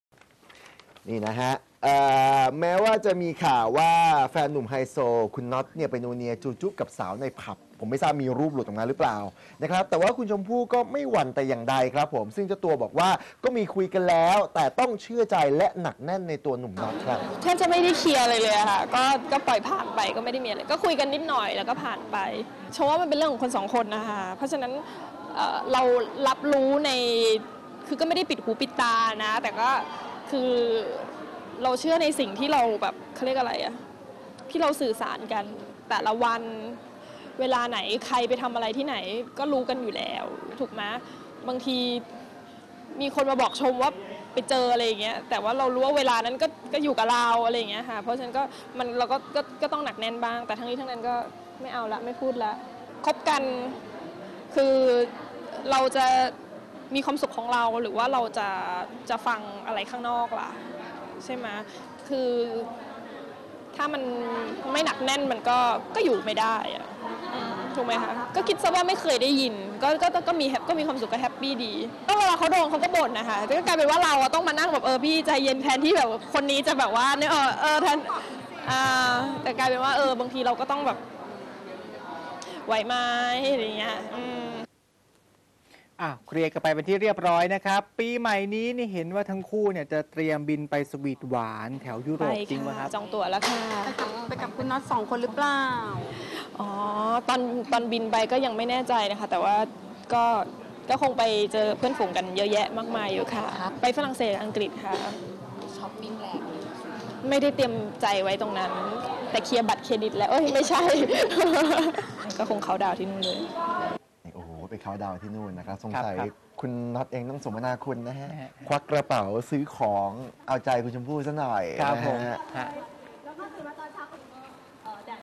Chompoo Araya: ชมพู่แพลนฉลองปีใหม่
True Inside News    19/11/2010
ชมพู่มั่นใจน็อตไม่นอกใจ  แพลนฉลองปีใหม่ที่ยุโรป